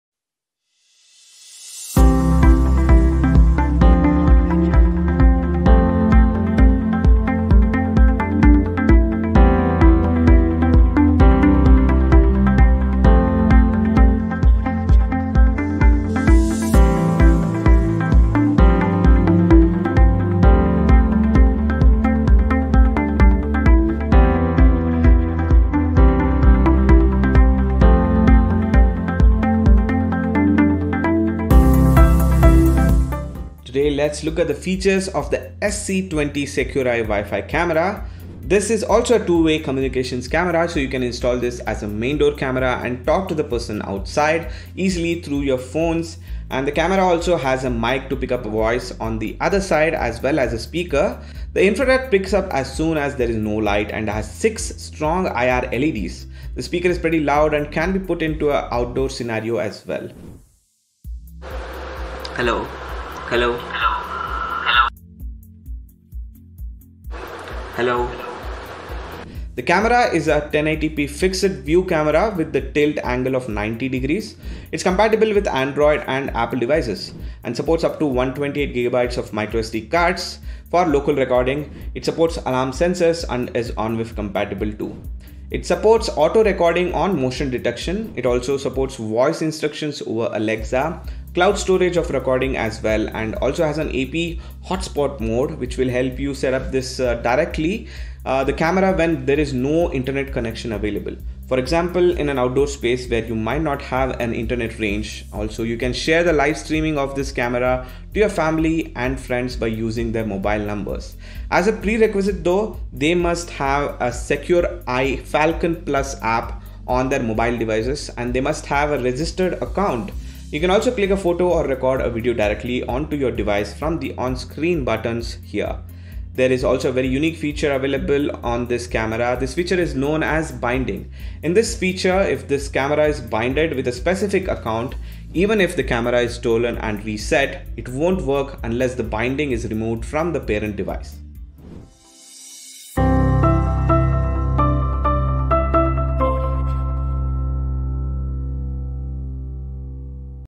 Secureye Wireless Camera S-C20 | Feature Explanation | Best Home Security Camera
Secureye Presents Wireless Camera with Two Way Communication which enabled to ensure the security of your home in your absence. You can view the live video footage on your mobile app. It is a feature-packed camera that is perfect for home security.